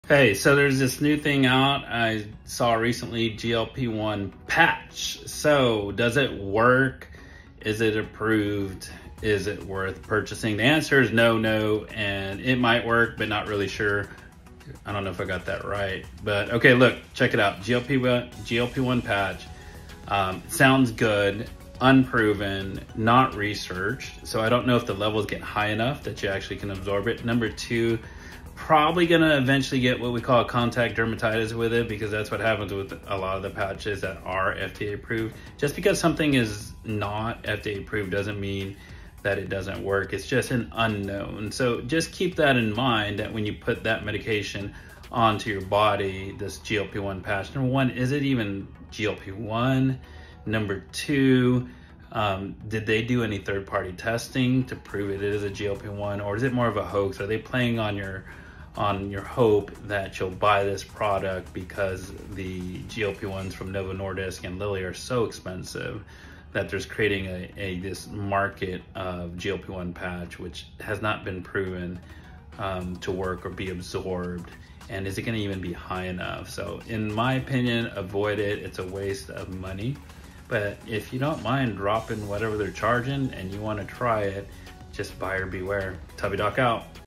Is the GLP-1 Patch Worth It? A Critical Review
GLP-1 Patch: Is It Worth Buying?
In this video, Tubby Duck investigates the new GLP-1 patch, questioning its effectiveness, approval status, and potential risks. He raises concerns about the lack of research, possible side effects like contact dermatitis, and whether the product is a scam aimed at capitalizing on the high cost of GLP-1 products from major pharmaceutical companies. TubbyDoc ultimately advises viewers to avoid purchasing the GLP-1 patch due to these uncertainties.